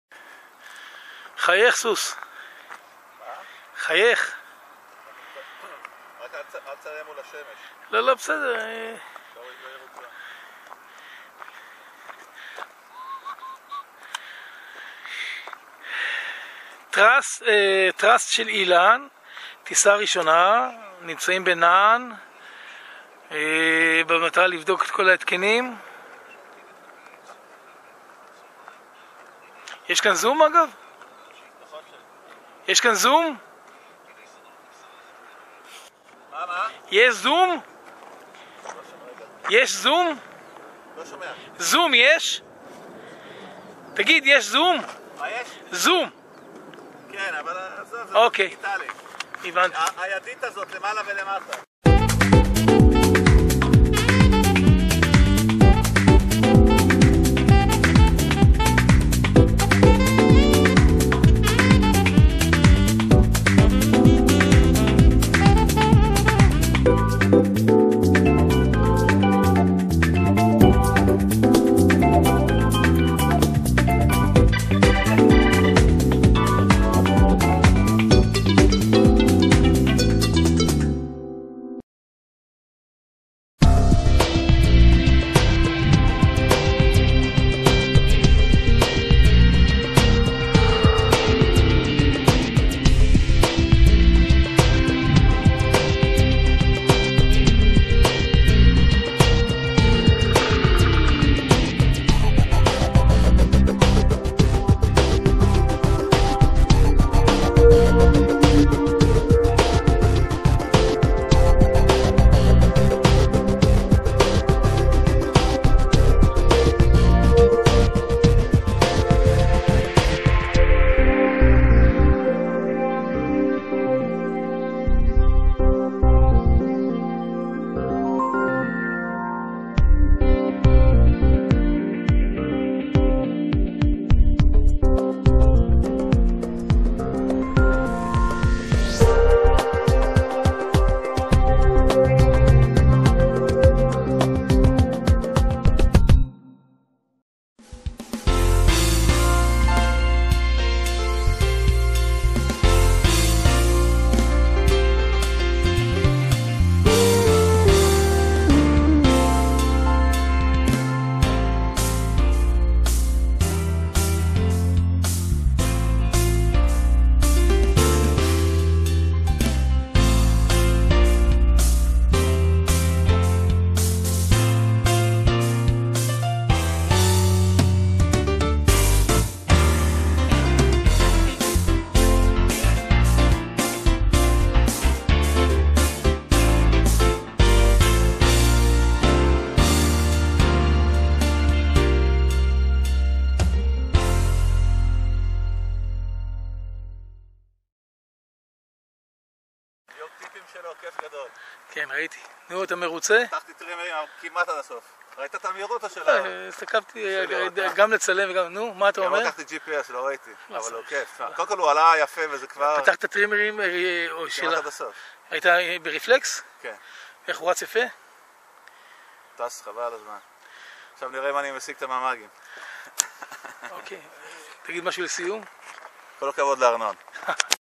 force large 9.11.11
my first flight on my force large paratrike